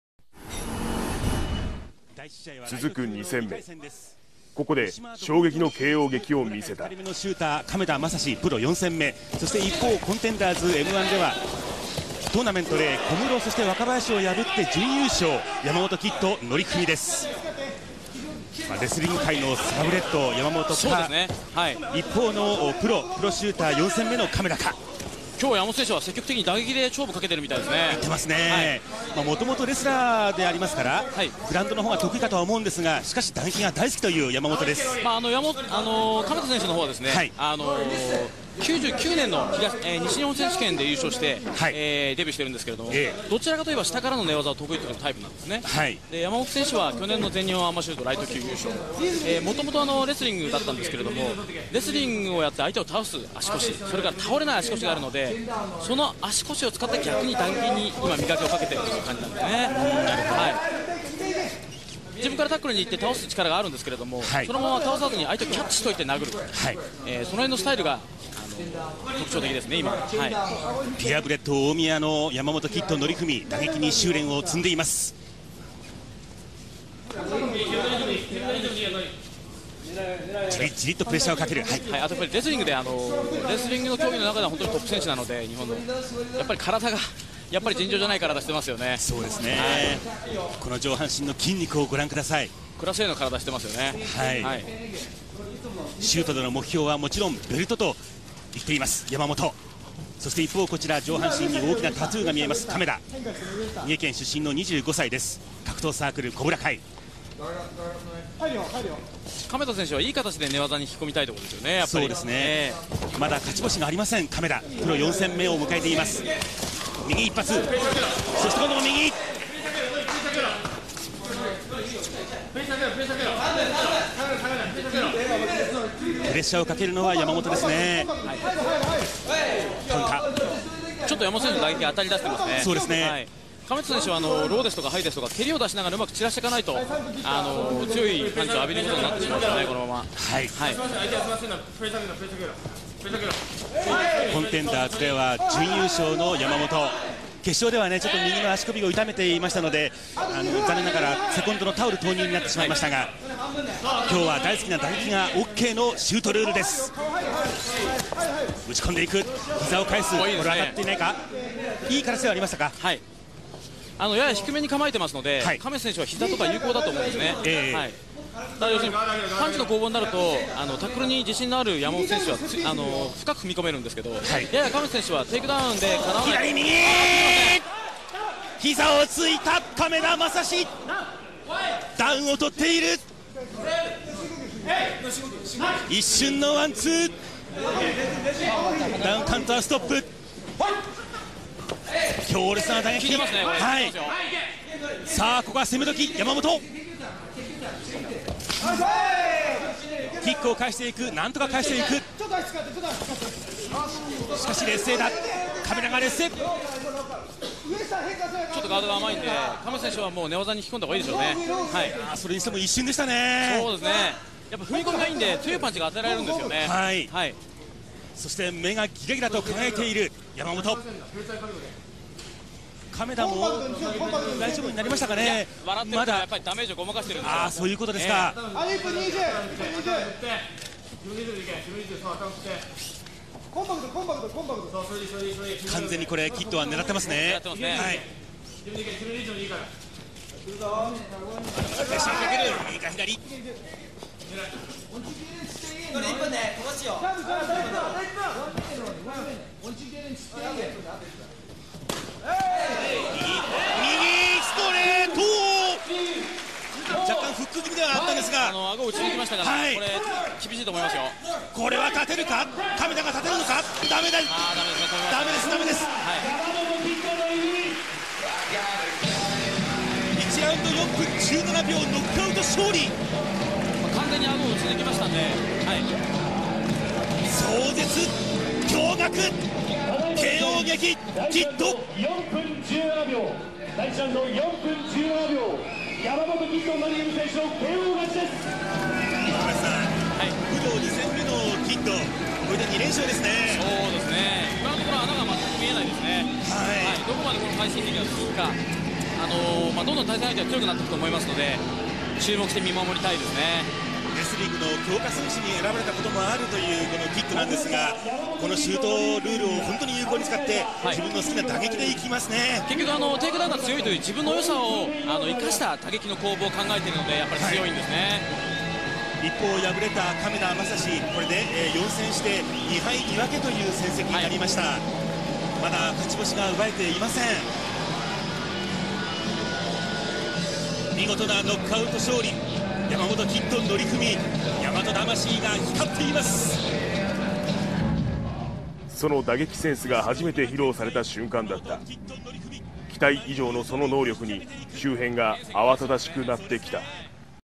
Norifumi KID Yamamoto vs Masashi Kameda
Yamamoto's 2nd Shooto bout against Masashi Kameda